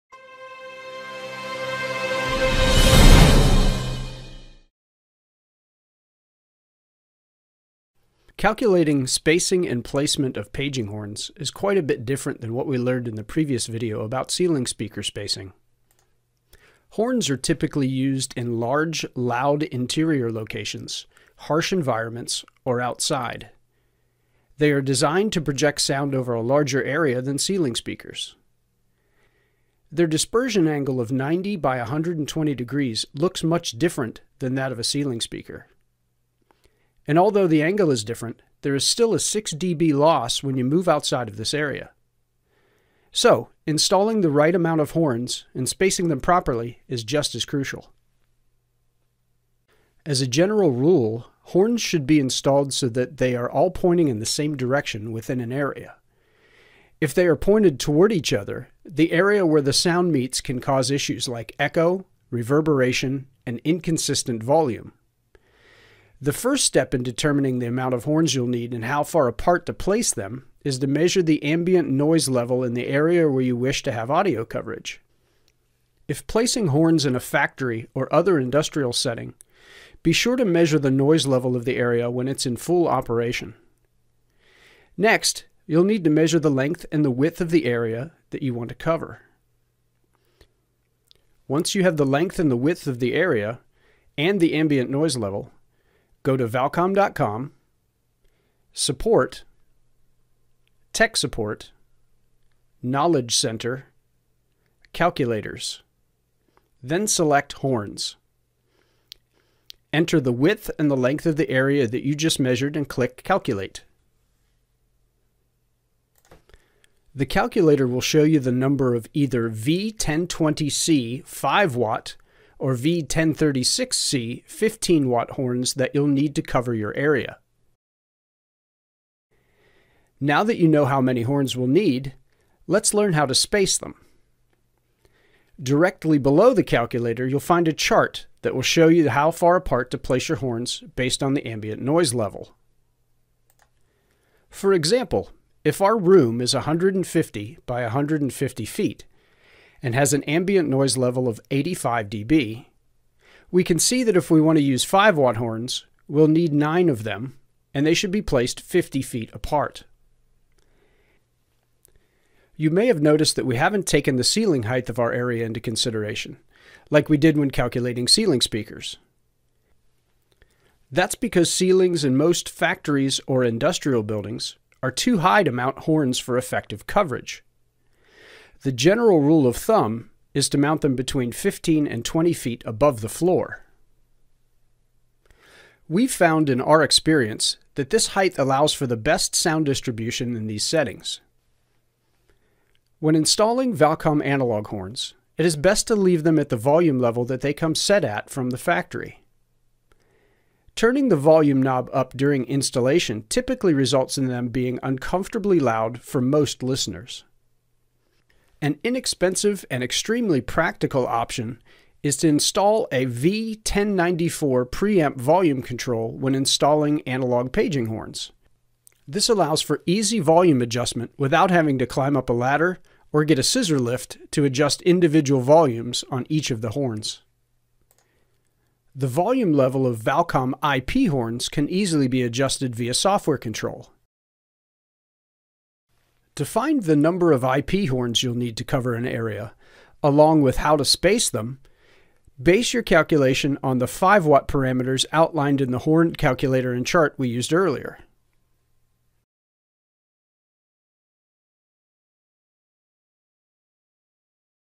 Layout Basics Part 3 - HORN PLACEMENT AND SPACING
This video explains the best practices for placement and spacing of Valcom paging horns.